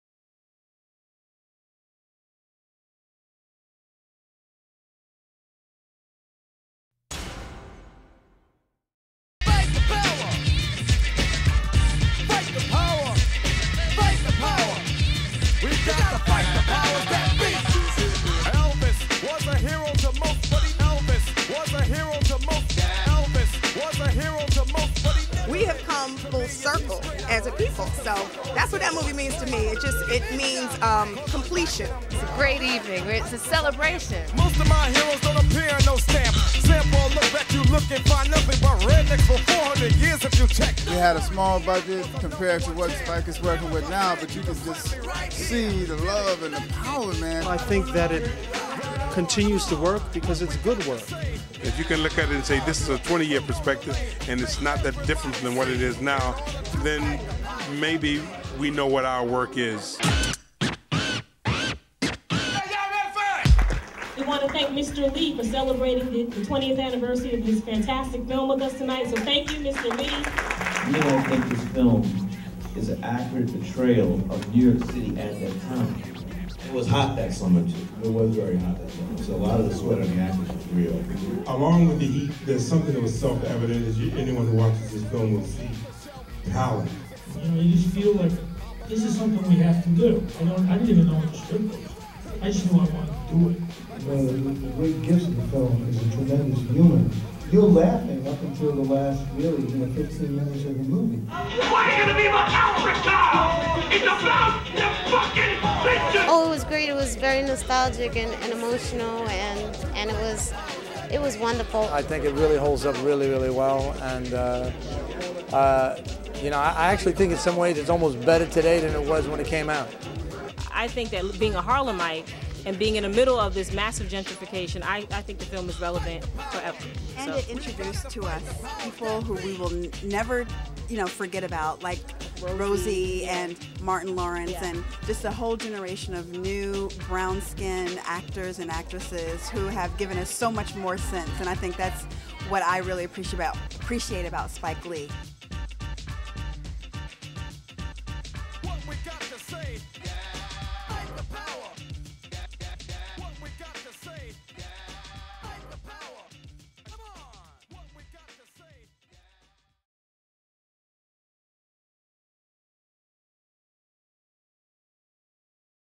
Do the Right Thing 20th Anniversary @ Lincoln Center
Spike Lee, Cast & Crew gather to celebrate the 20th Anniversary of Do The Right Thing.

Produced by Dominga Martin for Creme-Magazine.com, in comnjunction with Starz of WWW